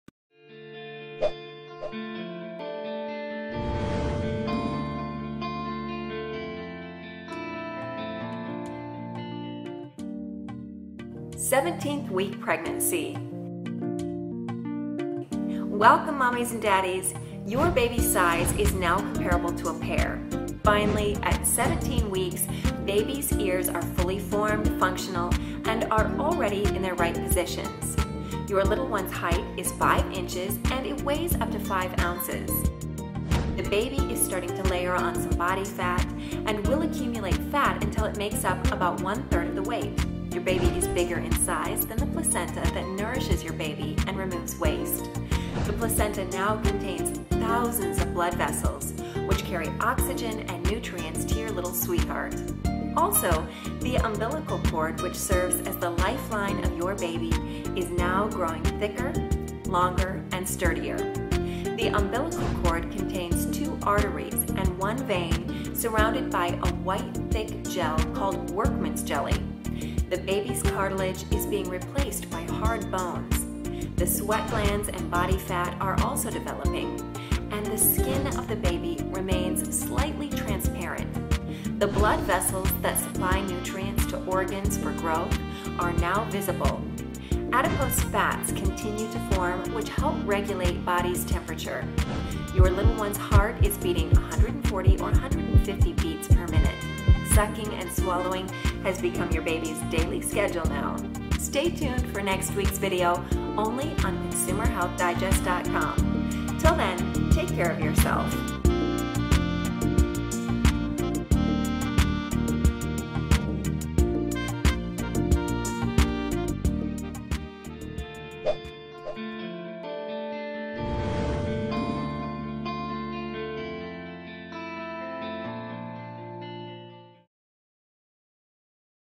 17 Weeks Pregnant: Watch Your Baby's Development
You are now 17 weeks pregnant. During this week, your baby’s umbilical cord gets thicker, lengthier and sturdier.

If you want to experiences a healthier, happier and beautiful pregnancy week.

From conception to birth, take a unique look on fetal development during the 9 months of pregnancy. Discover how your baby grows and what happens to your body during the whole pregnancy week.

Video Transcription:

Welcome Mommies and daddies

- your baby's size now is comparable to a pear
- Finally, at 17th week, baby's ears are fully formed, functional and are already in their right positions
- Your little one's  height is 5 inches and weighs up to 5 ounces
- The baby is starting to layer on some body fat and will accumulate fat until it makes up about one-third of the weight
- Your baby is bigger in size than placenta that nourishes your baby and removes waste
- The placenta now contains thousands of blood vessels which carries oxygen and nutrients to your little sweetheart
- Also the umbilical cord which serves as the lifeline of your baby is now growing thicker, lengthier and sturdier
- The umbilical cord contains two arteries and one vein surrounded by a white thick gel called workmen's jelly
- The baby's cartilage is being replaced by hard bones
- The sweat glands and body fats are also developing, and
- The skin of the baby remains slightly transparent
- The blood vessels that supply nutrients to organs for growth are now visible
- Adipose fats continue to form which help regulate body's -temperature
- Your little one's heart is beating 140 or 150 beats per minute
- Sucking and swallowing has become your baby's daily schedule now